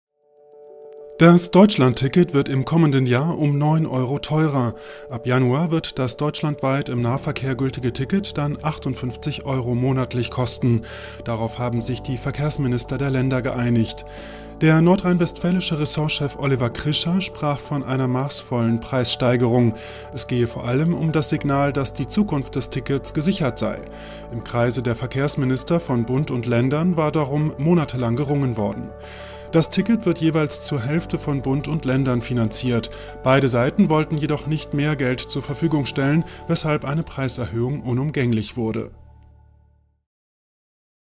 Preiserhöhung: Deutschlandticket kostet ab Januar 58 Euro
Der Preis für das Deutschlandticket steigt im kommenden Jahr um neun Euro. Ab Januar wird das deutschlandweit im Nahverkehr gültige Ticket 58 Euro monatlich kosten. Darauf haben sich die Verkehrsminister der Länder geeinigt.